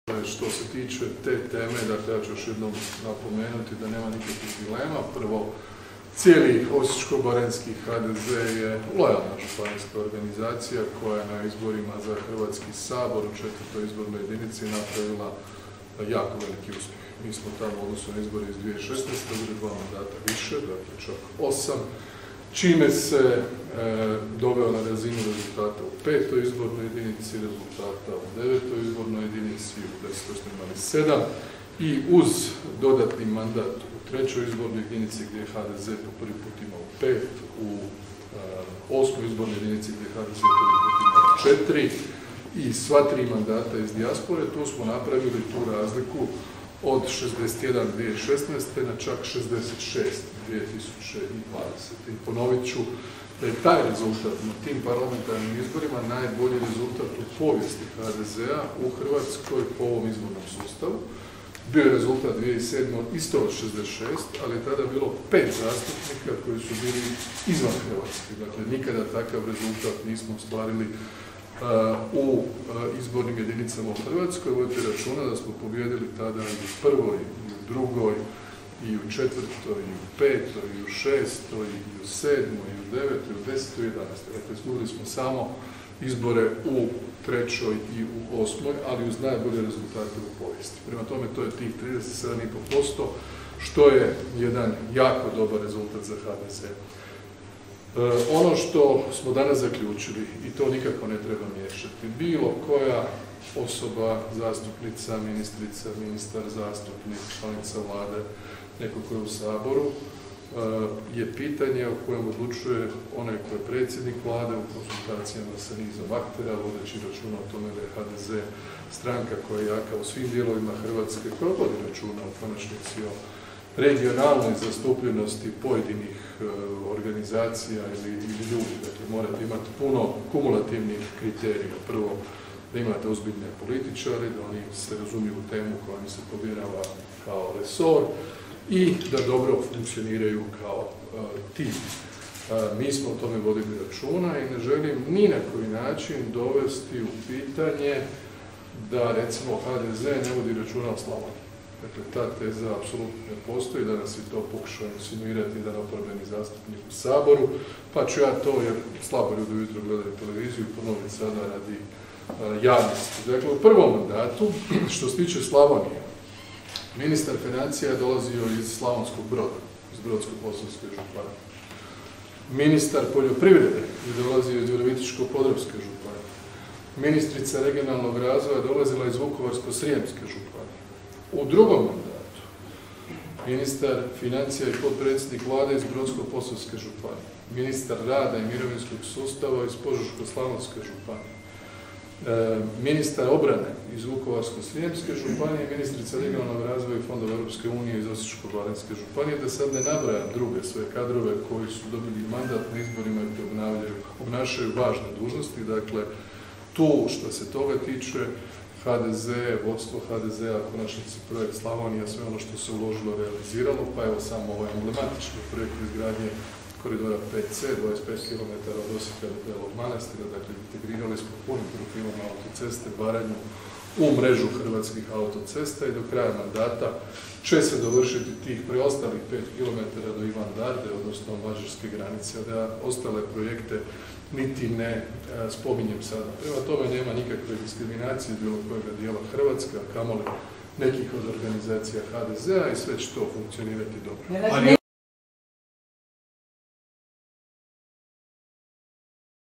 Plenković: "Cijeli slavonski HDZ lojalna je organizacija"
Konferencija za medije u središnjici HDZ-a nakon sjednice Predsjedništva HDZ-a. Medijima se obratio predsjednik Vlade RH Andrej Plenković.